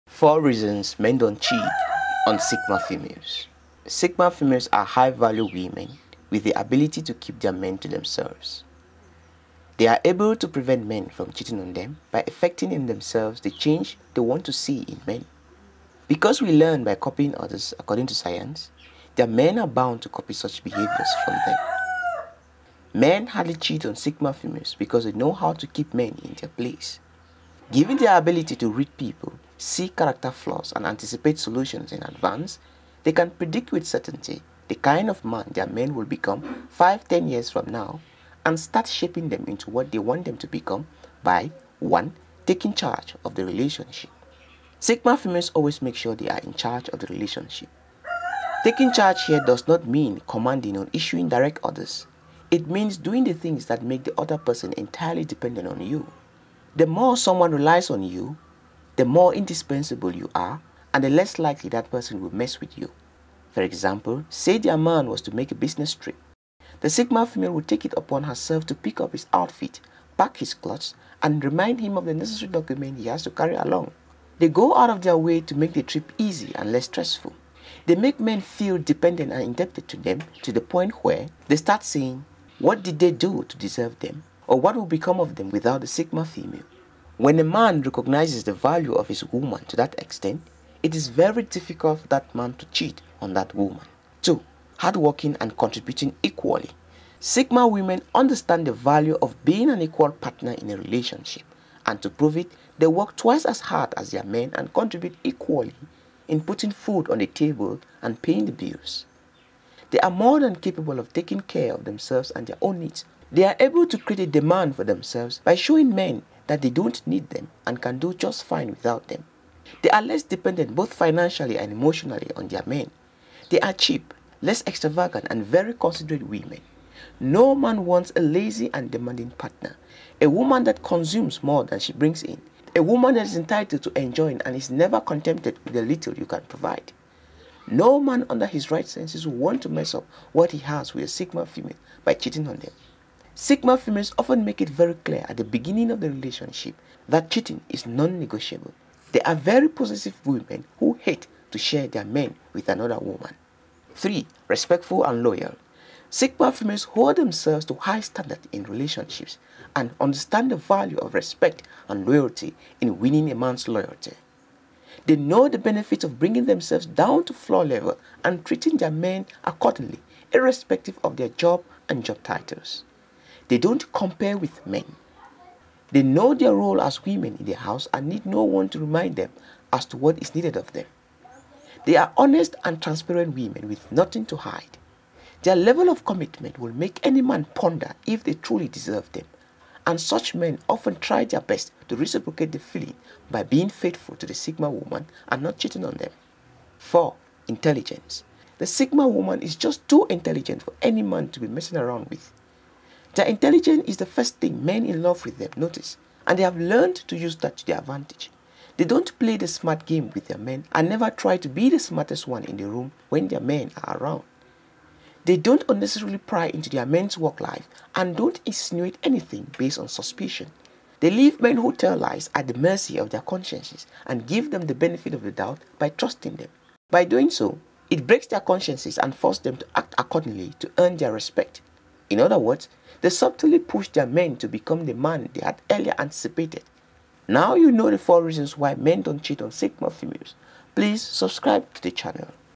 4 reasons men don’t CHEAT on sigma females
Sigma females are high value woman with the ability to keep their men to themselves. They are able to prevent men from cheating on them, by effecting in themselves they change they want to see in men. Because we learn by copying others, according to science, their men are bound to copy such behaviours from them.
Men hardly cheat on sigma females, because they know how to keep men in their place. Given their ability to read people, see character flaws and anticipate solutions in advance they can predict with certainty the kind of man their men will become 5, 10 years from now and start shaping them into what they want them to become by.